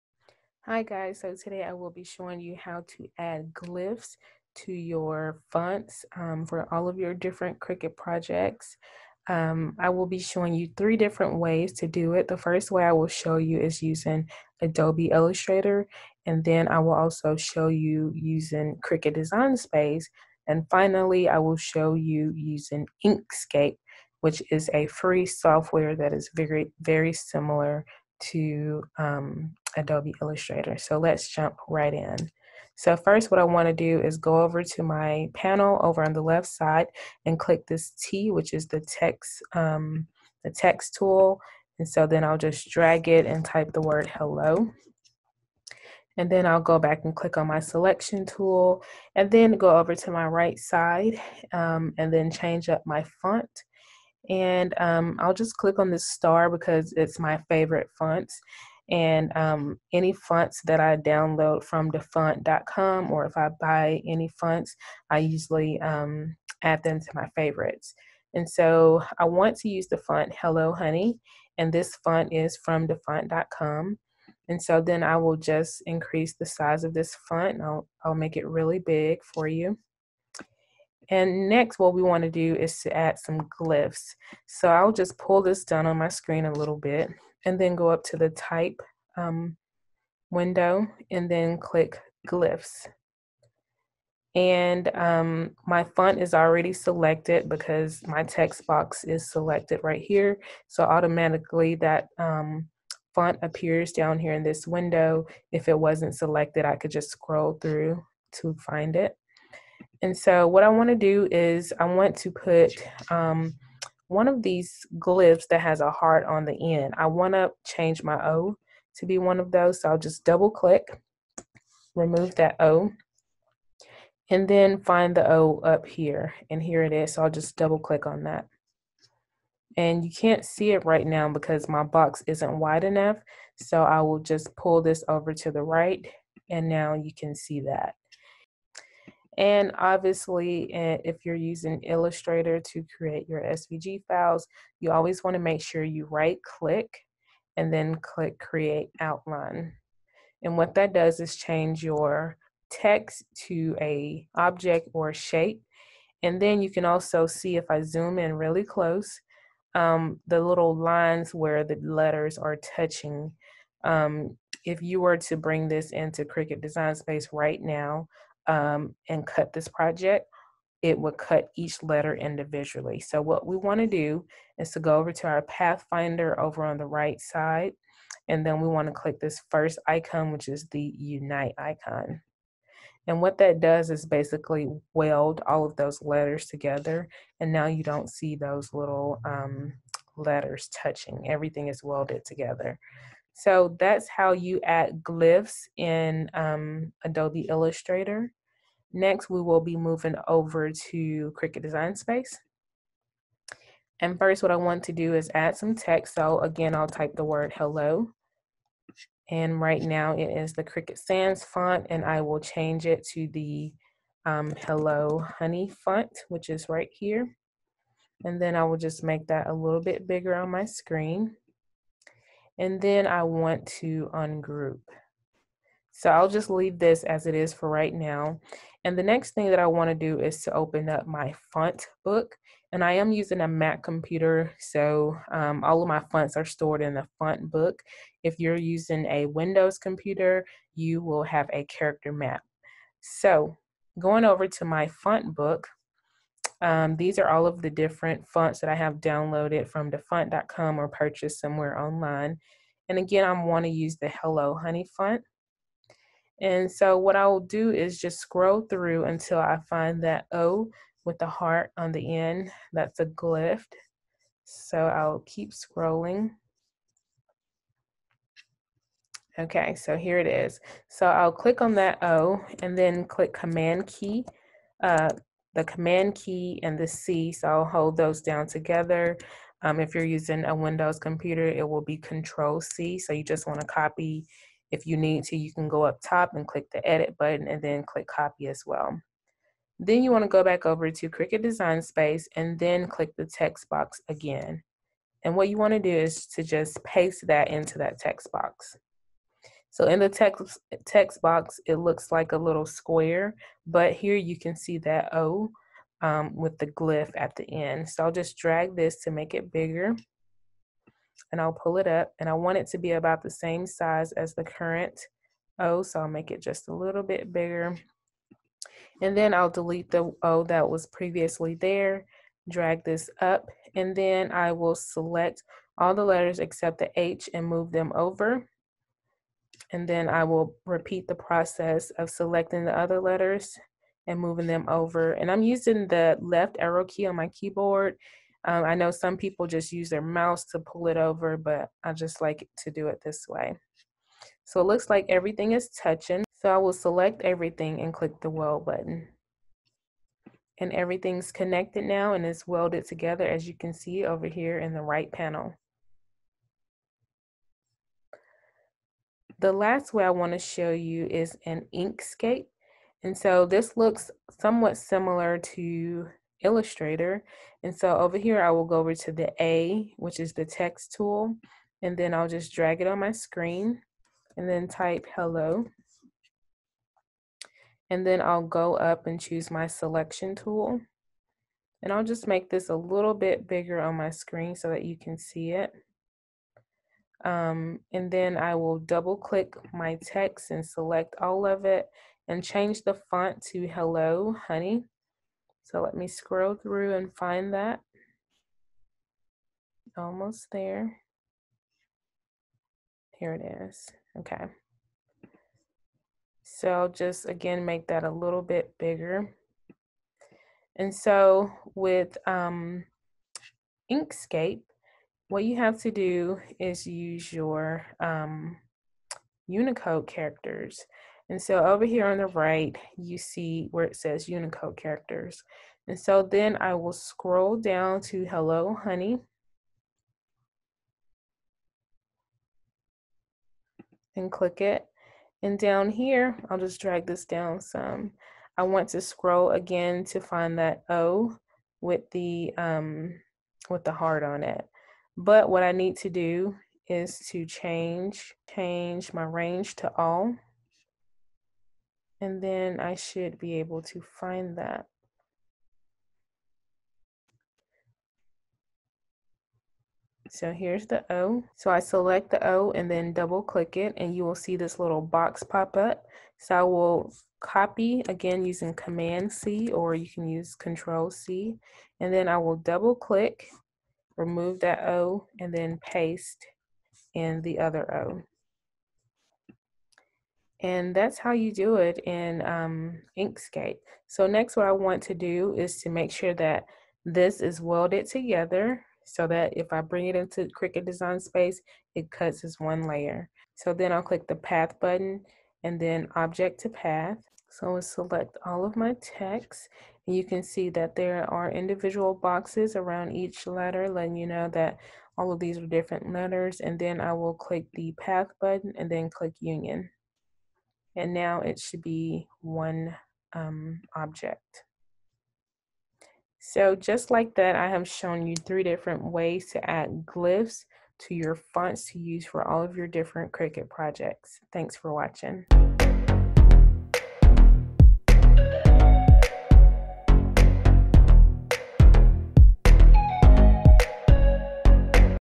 How to Add Glyphs and Flourishes to Fonts in Cricut Design Space (3 DIFFERENT WAYS)
In this video I show you how to easily add glyphs and flourishes to fonts in Cricut Design Space, Adobe Illustrator and Inkscape. All 3 ways work perfectly and is up to you which you prefer.

Schedule a private 1-on-1 Cricut session with me so I can show you how to use your Cricut machine and make a variety of projects

If you're using a Mac computer: You will use your Font Book if you want to add glyphs and flourishes directly to Cricut Design Space.

If you're using a Windows PC: You will use your Character Map if you want to add glyphs and flourishes directly to Cricut Design Space.

The font I used in this video is Hello Honey from Dafont.com

Each way takes less than 5 minutes to create your design.